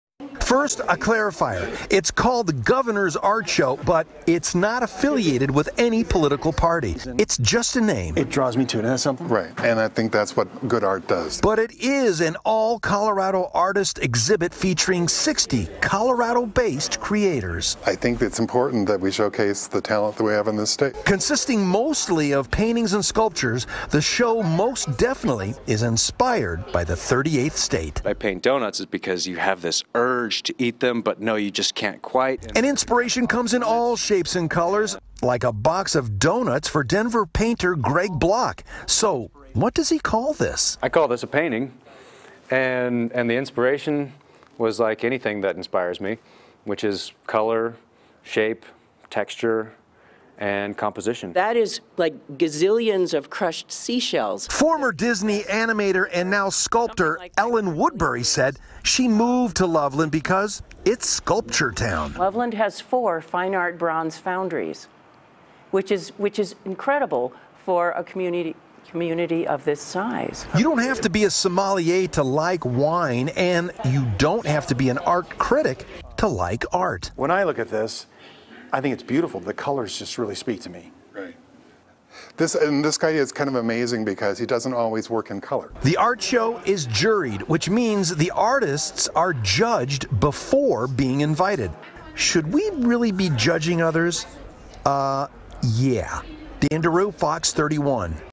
Governor’s Art Show gives Colorado artists space to shine
Looking for a chance to see some amazing art at a local art show that features only Colorado artists? The Governor’s Art Show is the largest art show in Colorado featuring only local artists.